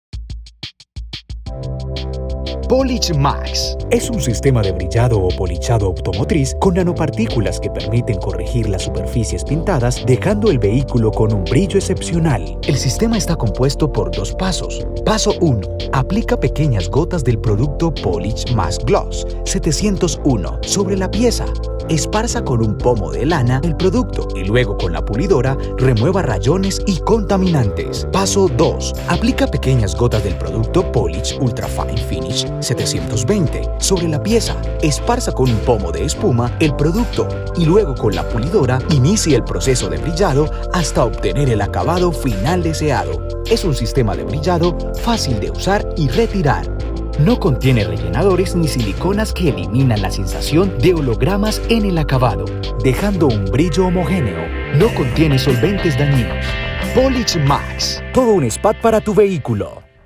Así puedes polichar tu vehículo con Polish Max de Pintuco
Aprende cómo aplicarlo y conoce todos los beneficios de esta solución Pintuco.
Este producto deja tu carro con un brillo excepcional.